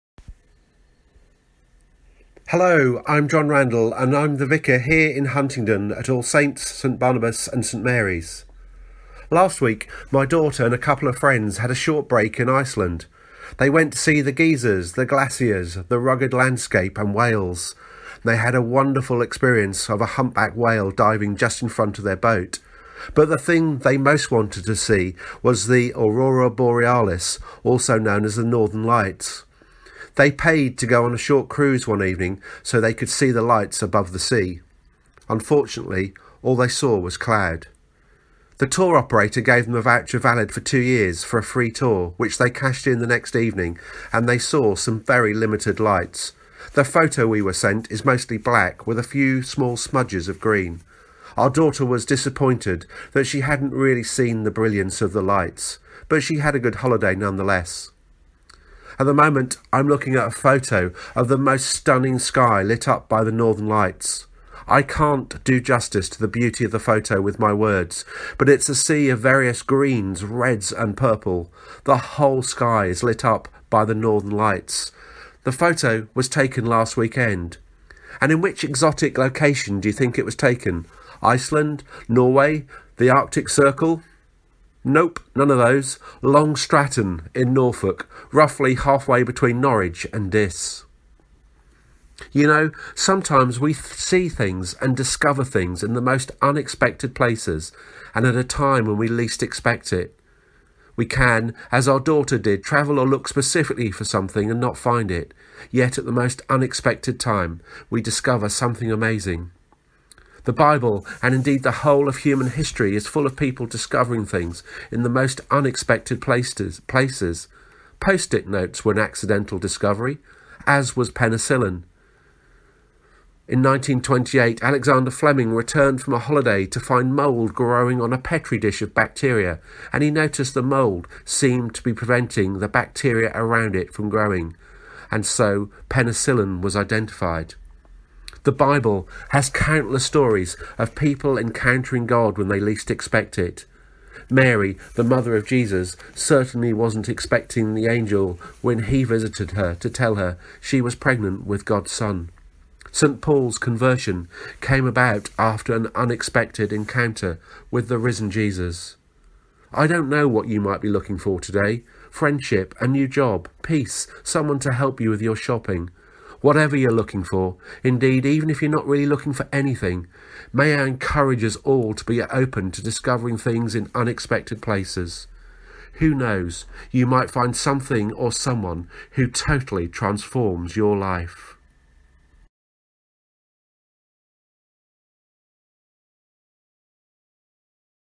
Sometimes we can find God in the most unexpected places
Sometimes we can find God in the most unexpected places. Listen to Jon Randall's reflection on this in this subtitled video. This was first broadcast on HCR 104 fm, our local radio station.🙌🤍